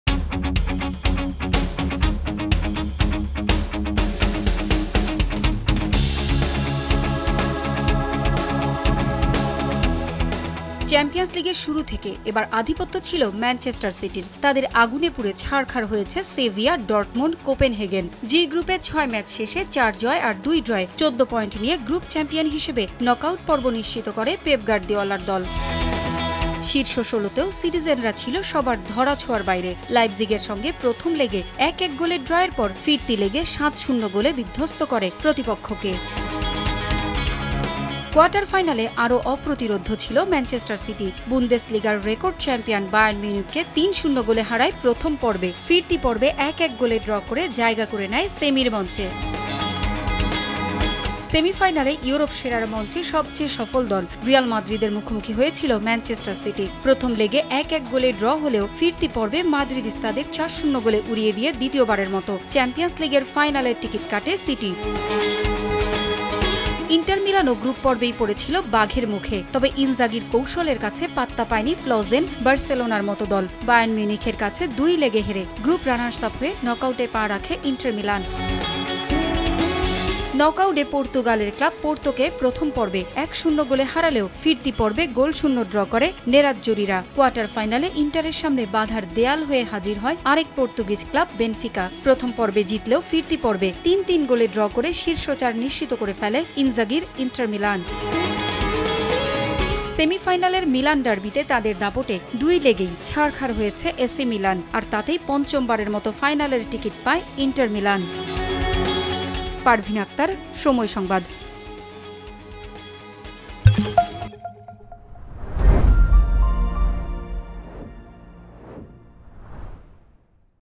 রাজার বেশে ফাইনালে ম্যানচেস্টার সিটি | Manchester City | Inter Milan | Somoy Sports
চ্যাম্পিয়ন্স লিগের শুরু থেকে এবার আধিপত্য ছিলো ম্যানচেস্টার সিটি। তাদের আগুনে পুড়ে ছাড়খার হয়েছে সেভিয়া, ডর্টমুন্ড, কোপেনহেগেন। জি গ্রুপে ৬ ম্যাচশেষে চার জয় আর দুই ড্র'যে ১৪ পয়েন্ট নিয়ে গ্রুপ চ্যাম্পিয়ন হিসেবে নকআউট পর্ব নিশ্চিত করে পেপ গার্িওলার দল।